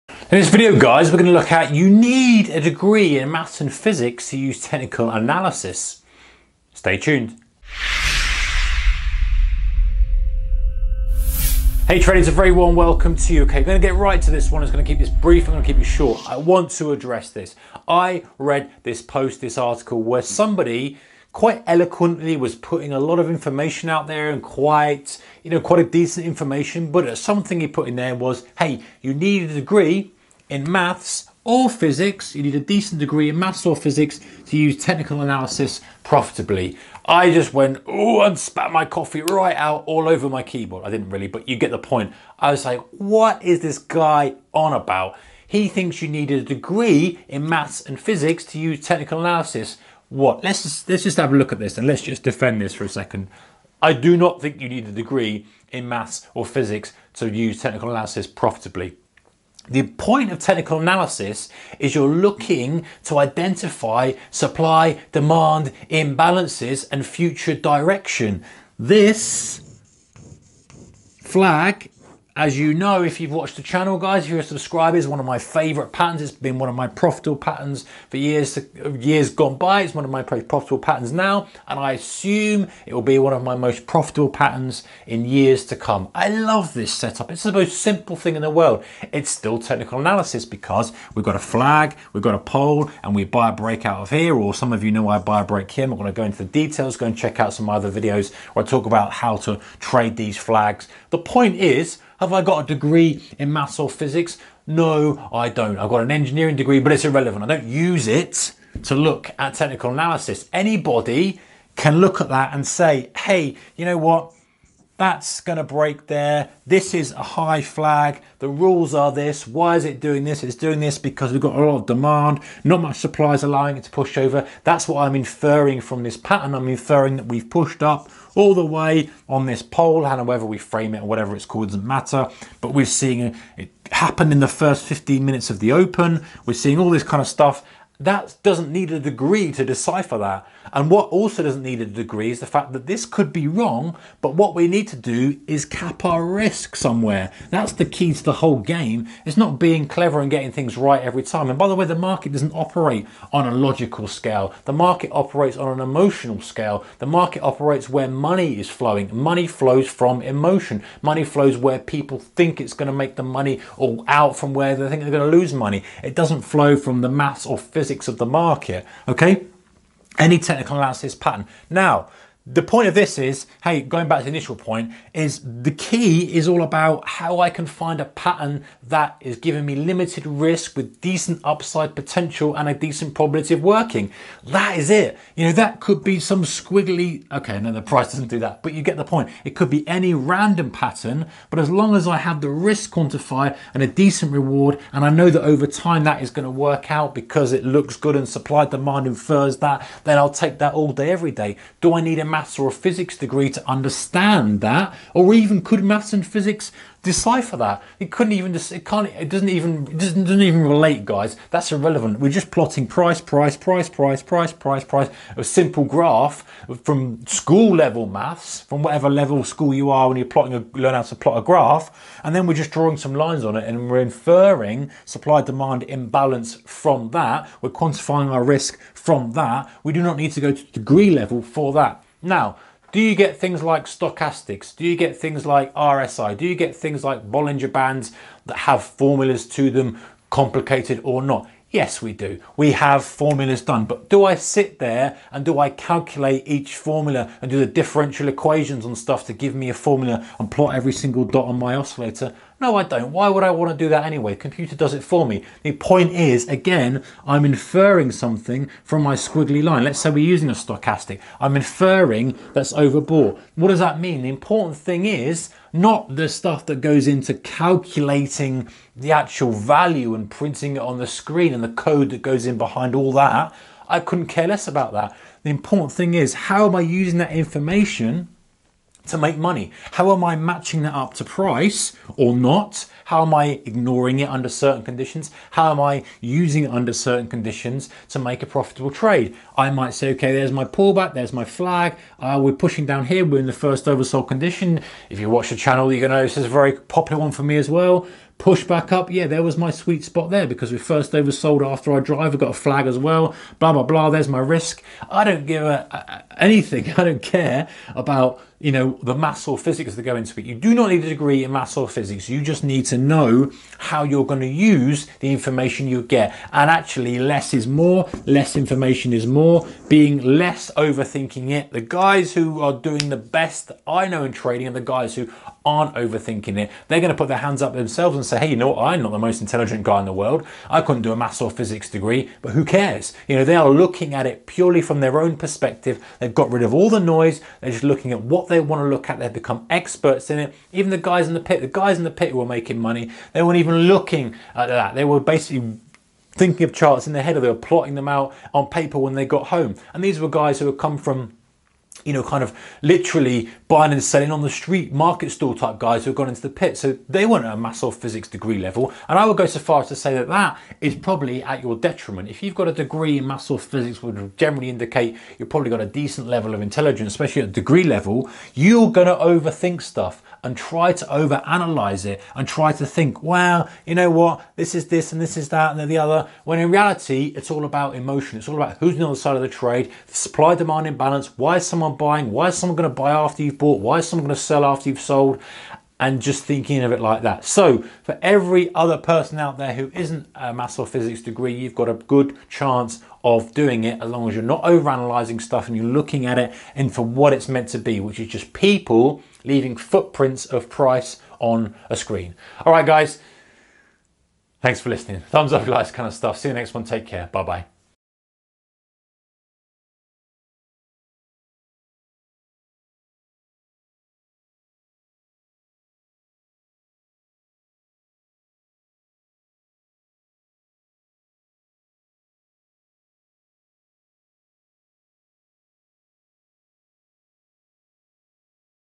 Is Stock Technical Analysis Profitable? Do you need a Degree? 🔢
Do you need a degree in Maths or Physics to use technical analysis profitably?  Does technical analysis really work? Is stock technical analysis profitable?  Can you make money with technical analysis?

I do not think you need a degree in Maths or Physics to use technical analysis profitably.  Technical analysis is all about identifying a supply/demand imbalance and future direction.
We make a prediction based on probabilities and then use risk management to cap our risk.
Do you get things like RSI, stochastics or Bollinger Bands that have complicated formulas behind them? No but we don't need to calculate the formulas each time we want to use this analysis; computers do it for us.
The key is how are we using this information to make money?  In most cases less is better and overcomplicating things doesn't help..